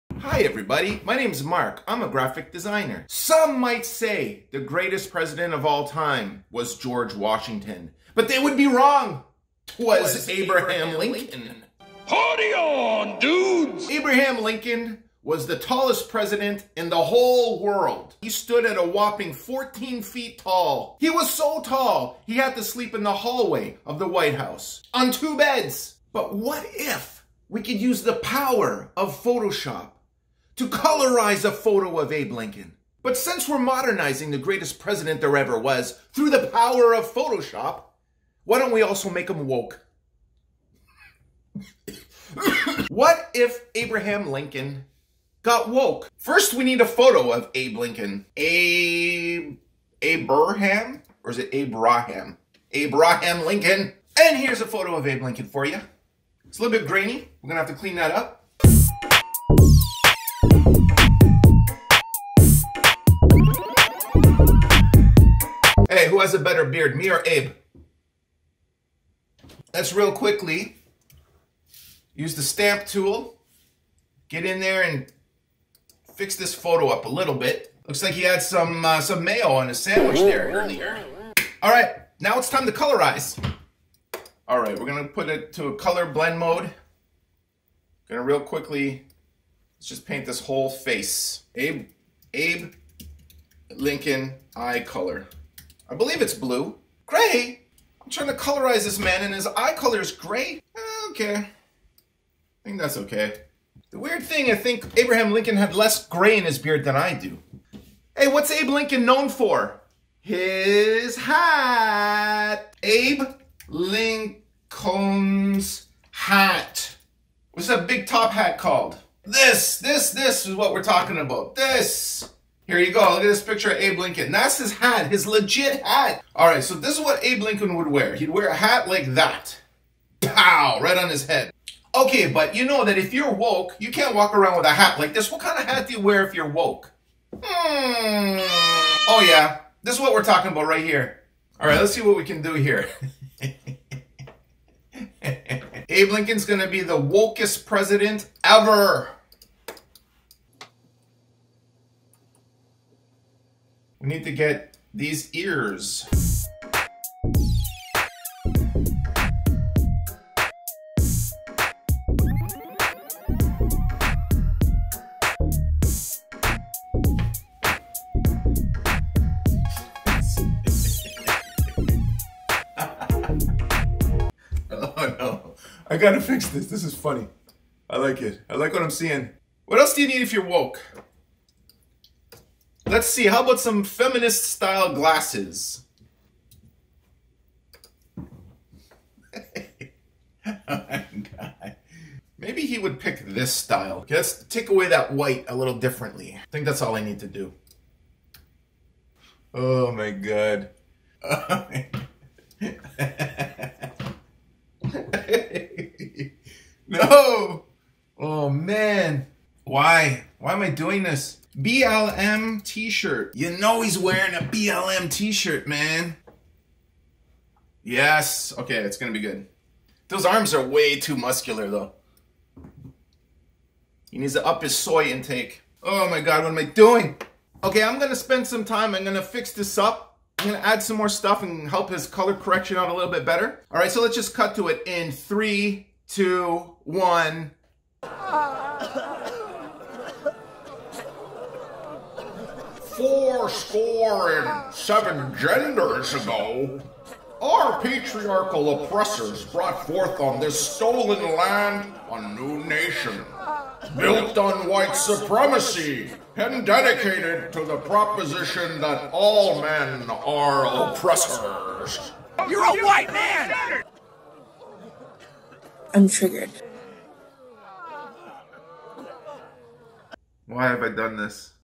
How to Colorize a B&W Photo using Photoshop!
I made the decision to colorize a black and white photo of President Abe Lincoln… and then I made him extremely WOKE! Lord have mercy on my soul.